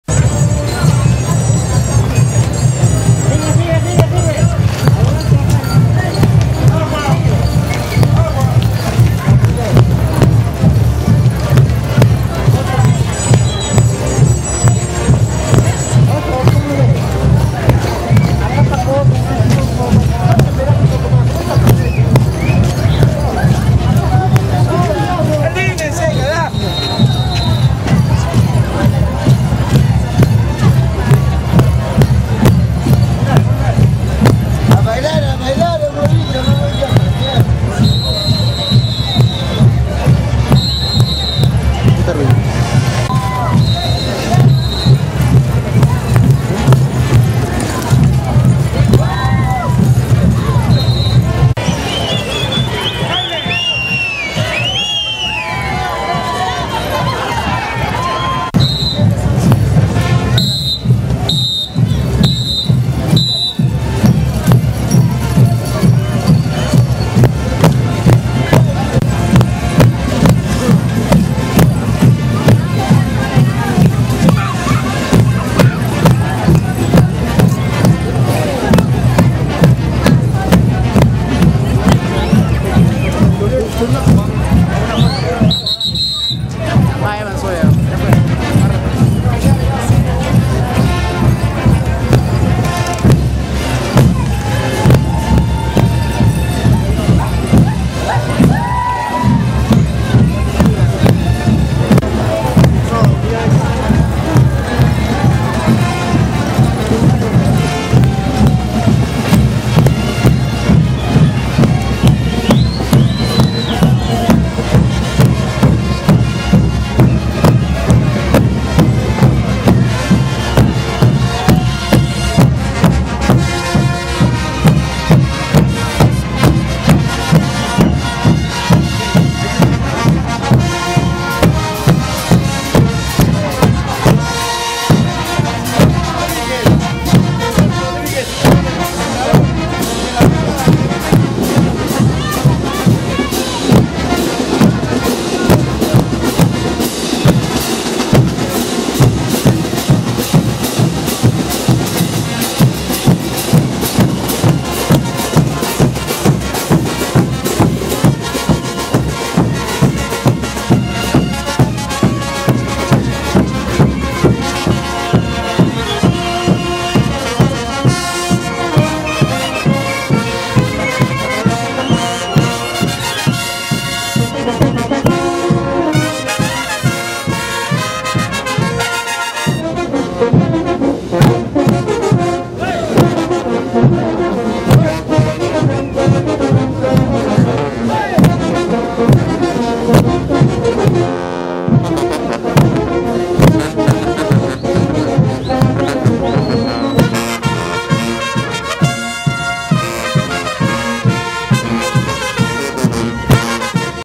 Bloque Chascosos y Amistad - Espectacular Diablada Bellavista 2013
Presentacion de la banda REAL MAJESTAD DE JULIACA en la CANDELARIA DE PUNO 2013